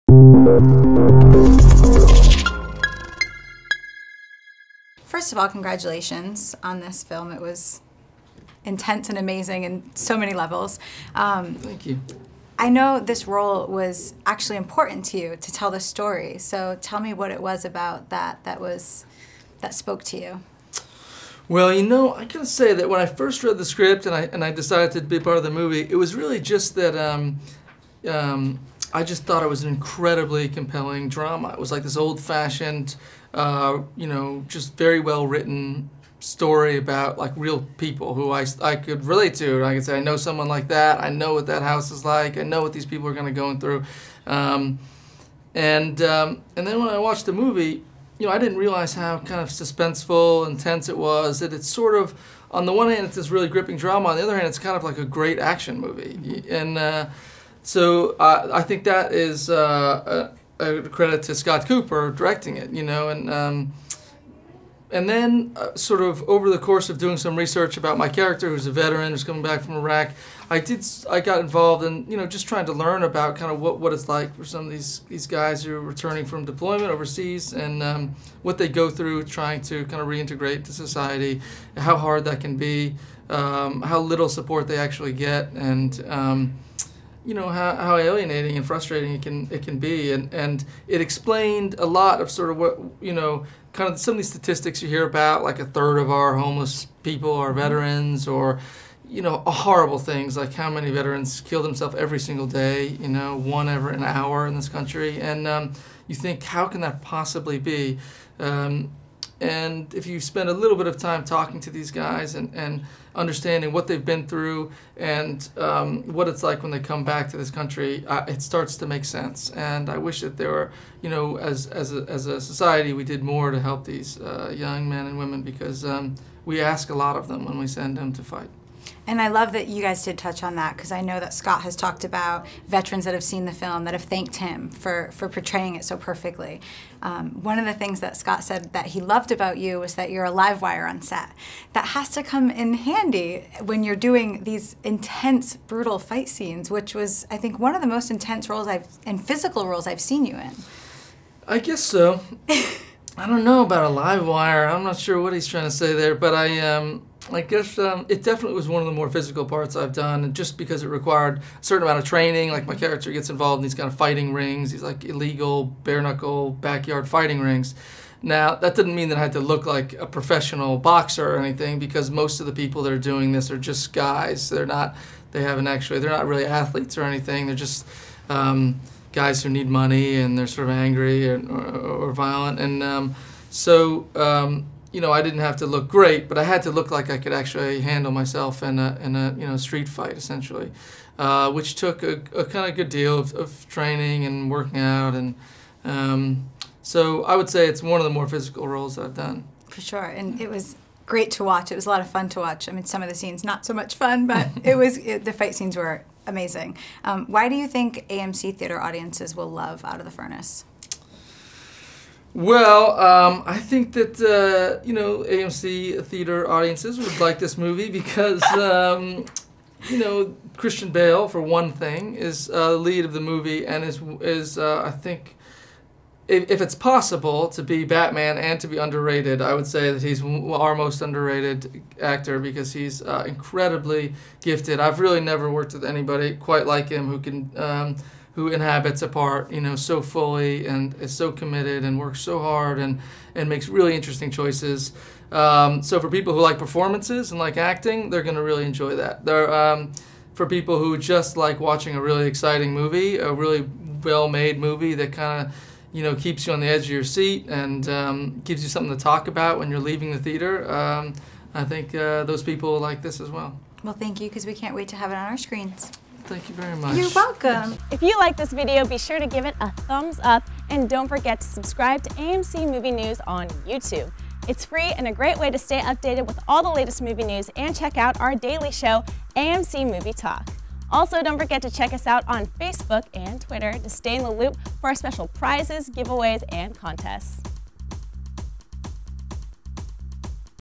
Casey Affleck Talks OUT OF THE FURNACE With AMC
Casey Affleck sat down with AMC's Krisily Kennedy to talk about his upcoming film OUT OF THE FURNACE which opens up in AMC Theatres everywhere December 6, 2013.